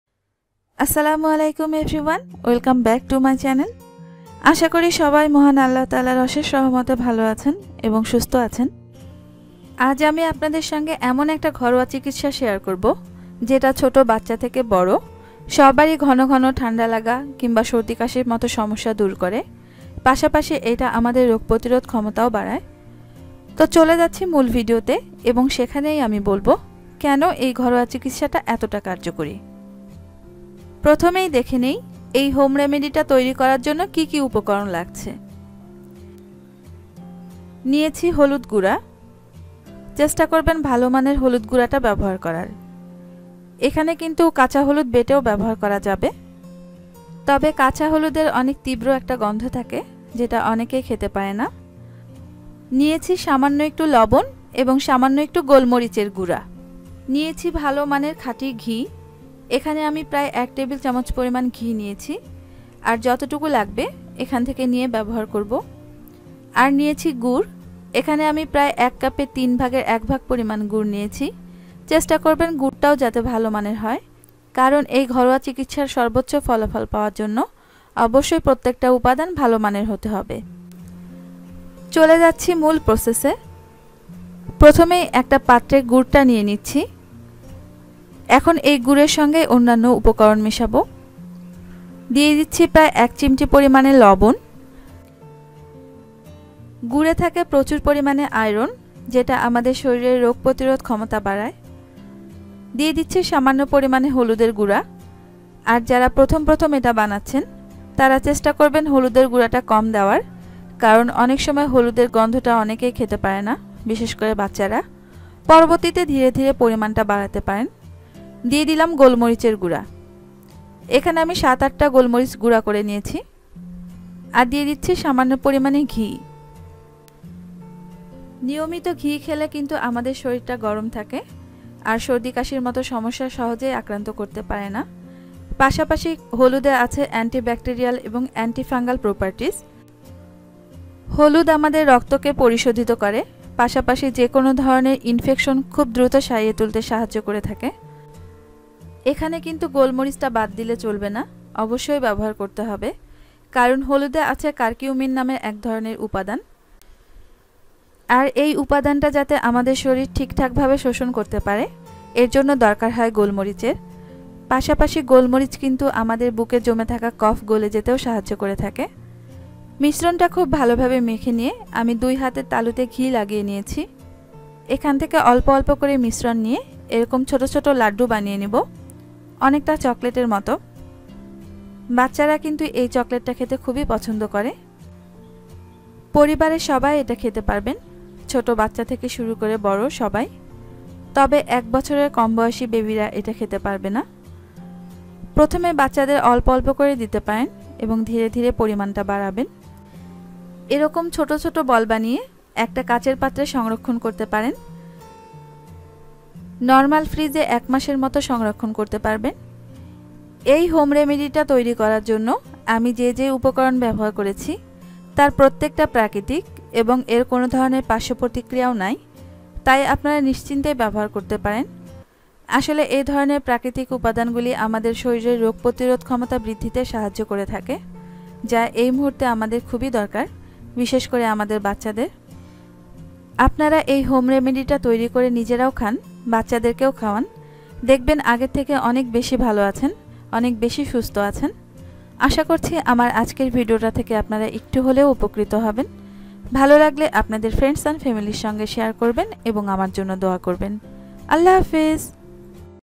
সর্দি কাশি : ঠান্ডা দ্রুত নিরাময়ে | রোগ প্রতিরোধ ক্ষমতা বাড়াতে | Remedies for baby Cough & Cold
বিশেষজ্ঞদের মতে সর্দি কাশির কোন ঔষধ ৪ বছরের কম বয়সী বাচ্চাদের জন্য নিরাপদ নয়। এসব ঔষধের পার্শ্বপ্রতিক্রিয়ার ফলে বাচ্চাদের জীবন হুমকির মুখে পরে। সুতরাং এসব ঔষধের পরিবর্তে নিরাপদ ও কার্যকর ঘরোয়া চিকিৎসা ব্যাবহার করতে পারেন।খুব সহজেই বাসায় বসে অল্প উপকরন দিয়ে তৈরী করে নিতে পারেন ১০০% কার্যকরি কোন ধরনের পার্শ্বপ্রতিক্রিয়া মুক্ত হোম রেমেডি ।
প্রয়োজনীয় উপকরন -

গুড় - ১/৩ কাপ
হলুদ গুড়া - ১/৪ চা চামচ
গোল মরিচ গুড়া - (৭-৮) টা
লবন - ১ চিমটি
ঘি - পরিমান মত